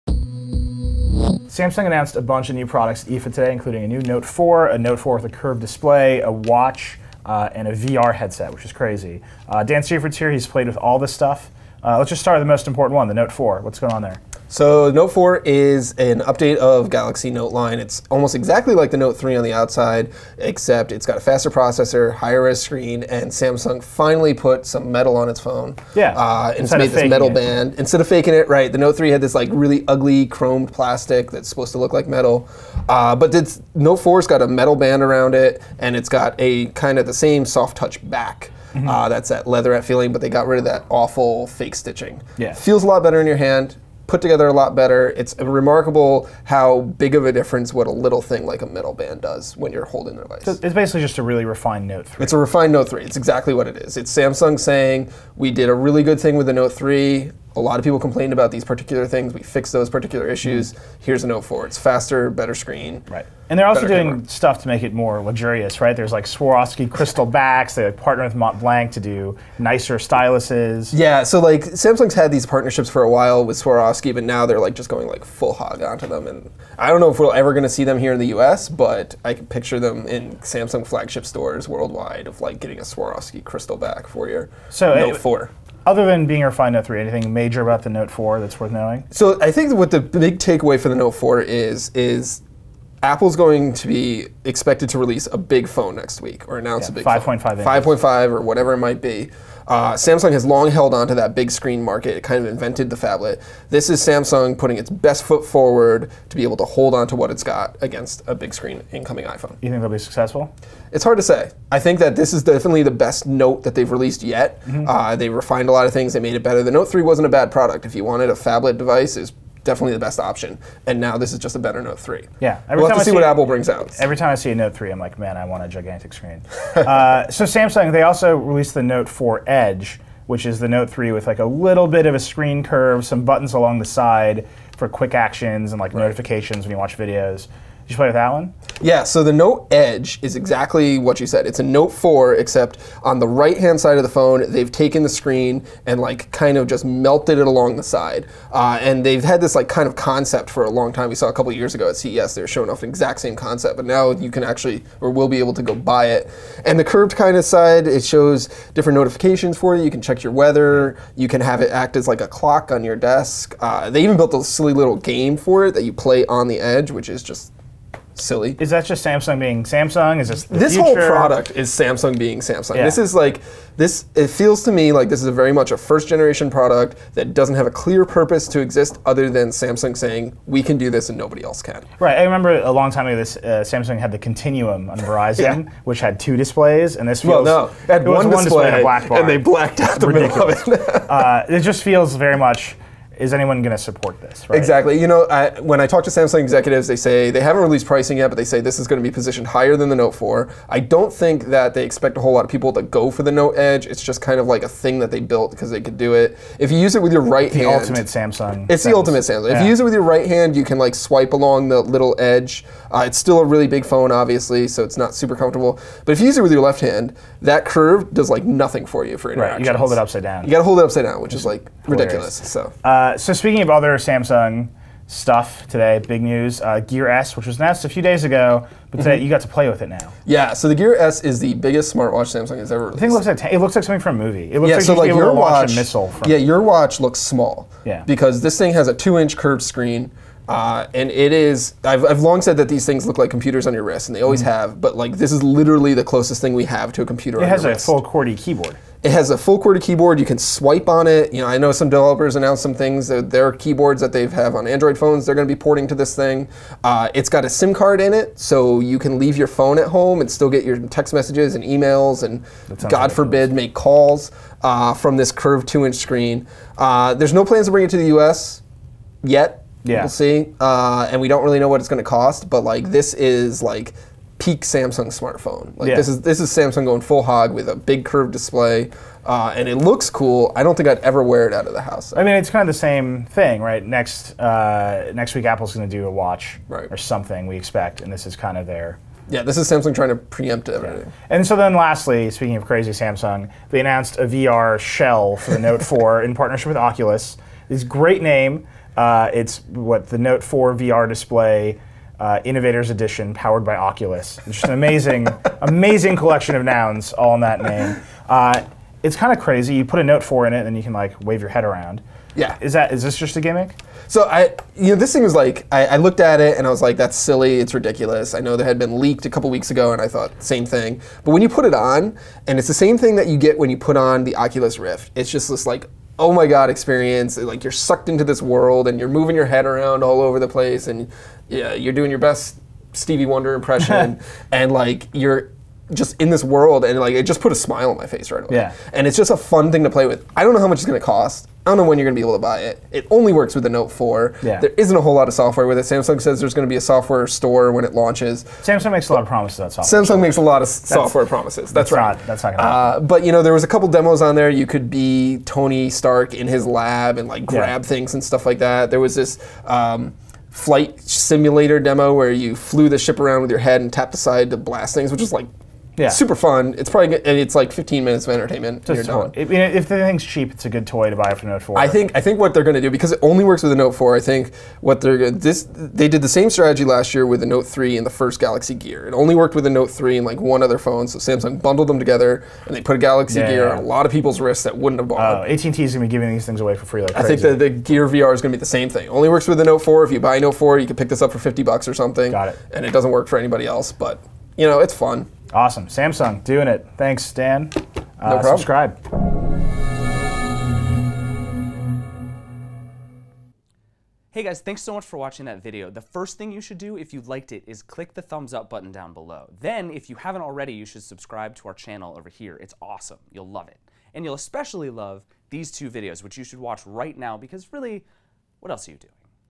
Why Samsung was the biggest thing at IFA 2014
Samsung announced the Note 4, the Note Edge, a VR headset, Swarovski crystal battery doors, and Mont Blanc styluses. Because Samsung isn't afraid to be a little crazy. Nilay Patel and Dan Seifert run through all the big news.